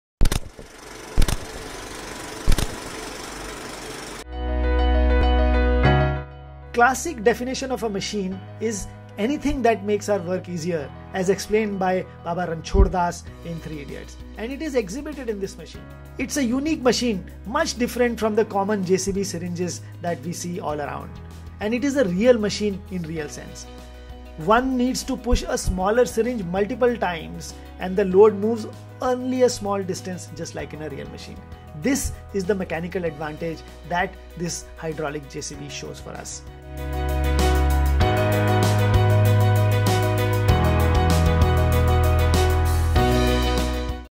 HYDRAULIC JCB REAL MACHINE
Classic definition of Machine “Anything that makes our work easier”  is exhibited int his machine. Unique and different from the common syringe JCB this machine is Machine in real sense.

One needs to push smaller syringe multiple times and then the load moves for a small distance. This Mechanical Advantage , must for any machine is achieved by introducing multiple values at various locations and different sized syringes.

Must for any student studying machine and not the textbook definition